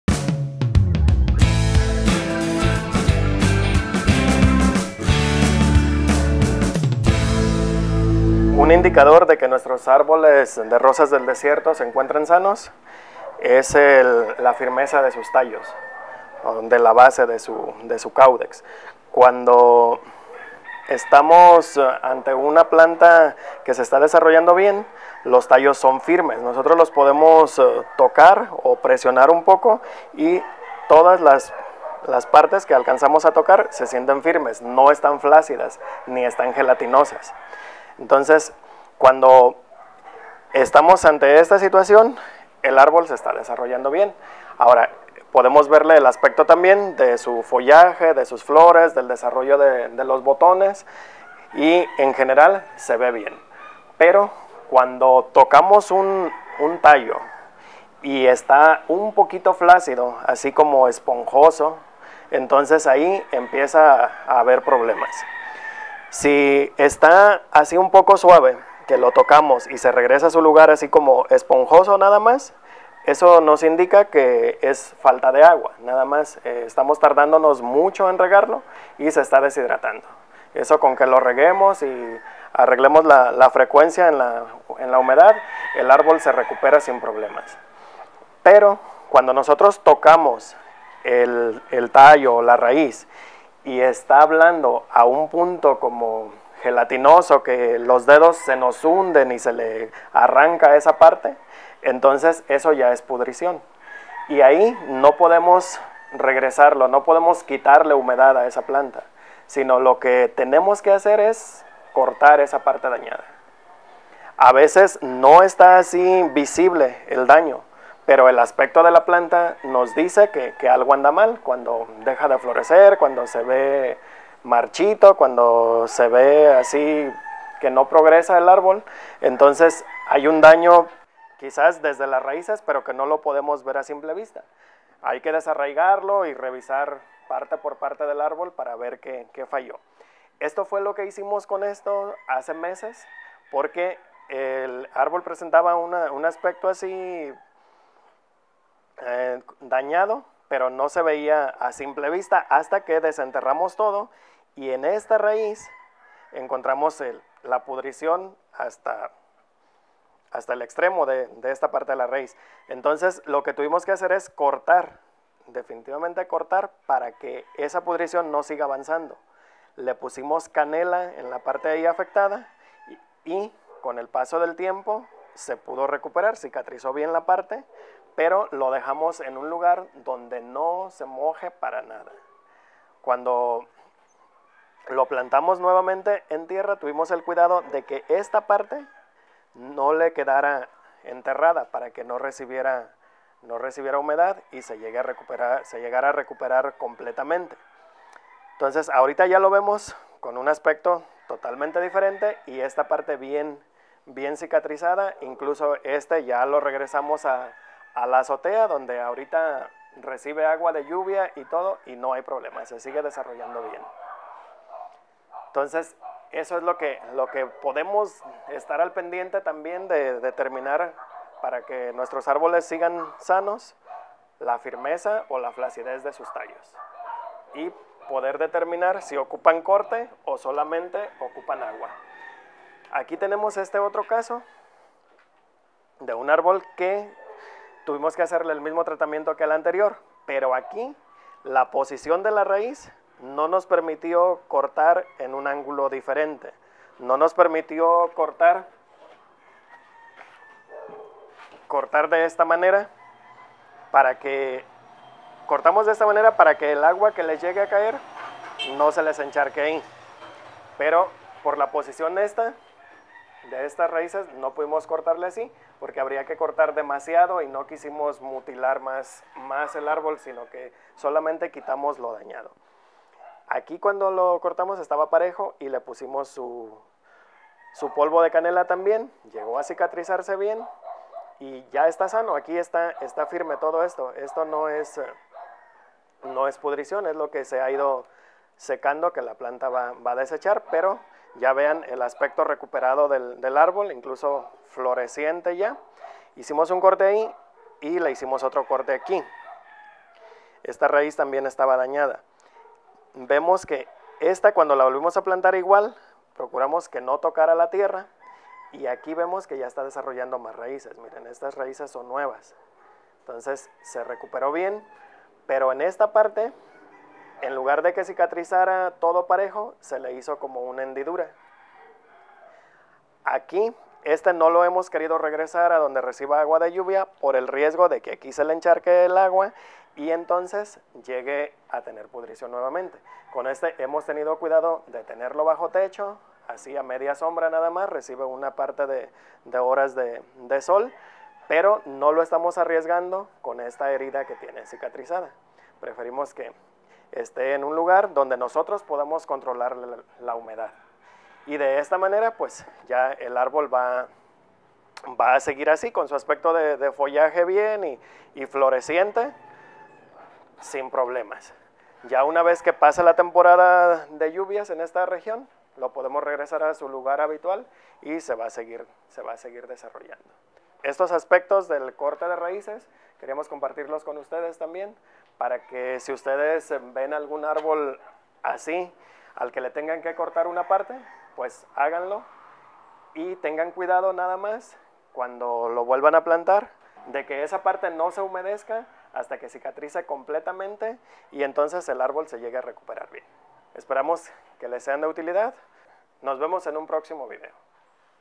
Como corregir pudrición en las raíces de Rosas del Desierto
Ante el exceso de humedad en las rosas del desierto no podemos hacer otra cosa que cortar las partes dañadas, poner algún cicatrizante y esperar a que se recuperen. En este video te explico como lograrlo.